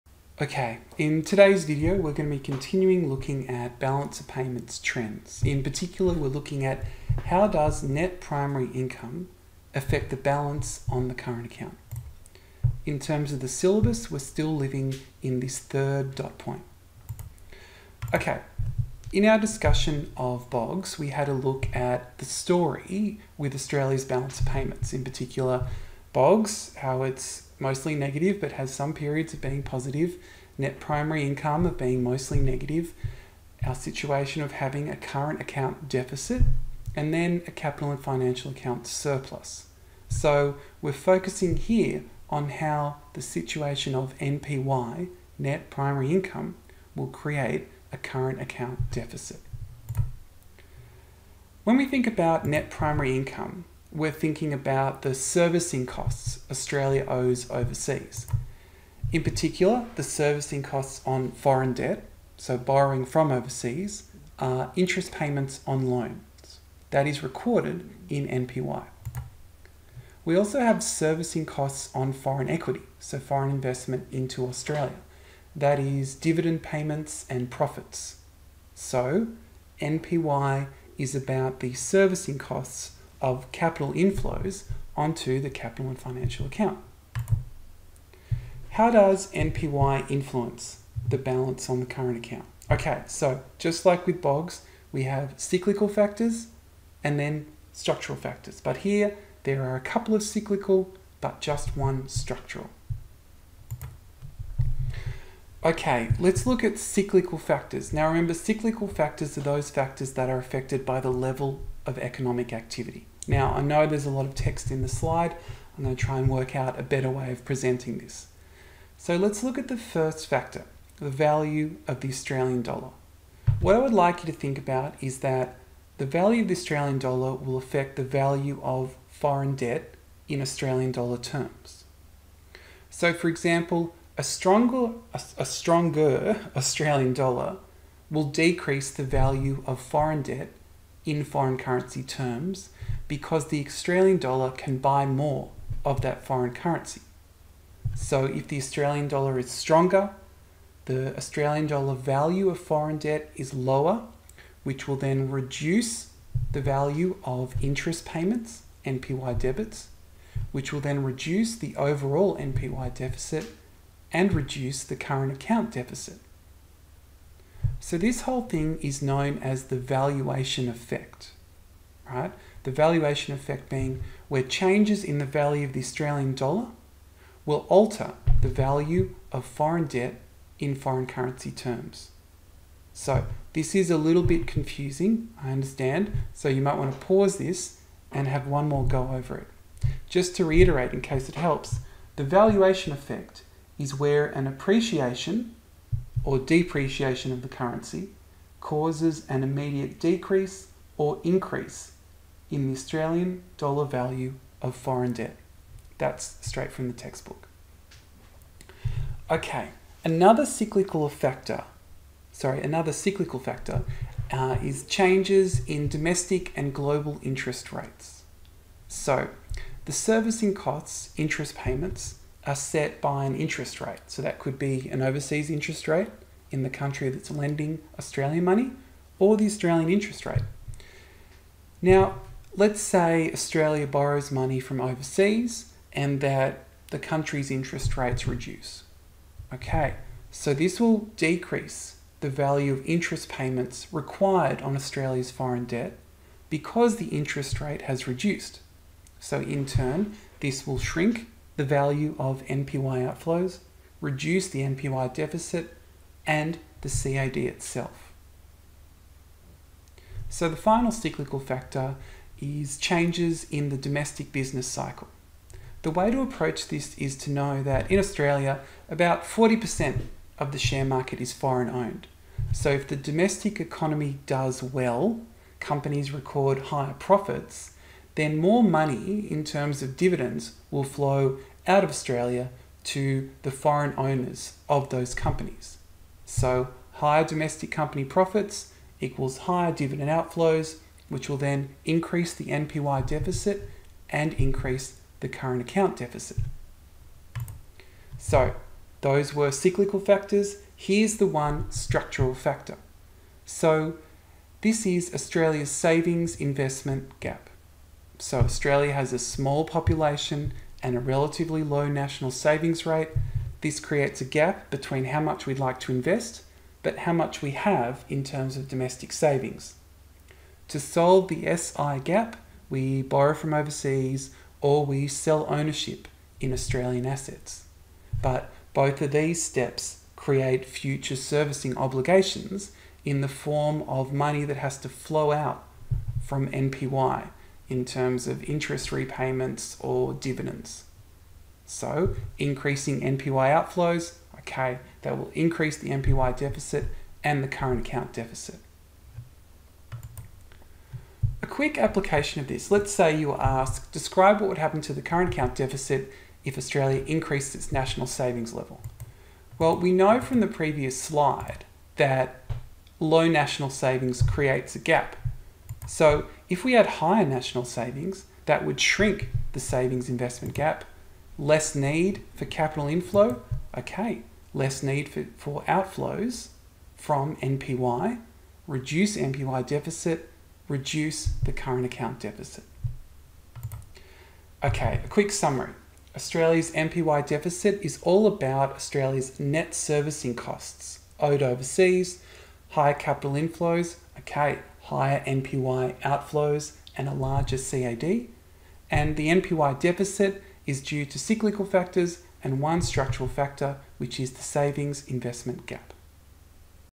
How changes in Net Primary Income affects the CAD
This video describes how changes in NPY contributes to a larger CAD, as part of the Balance of Payments. This relates to the NSW Economics HSC Course, Topic Two.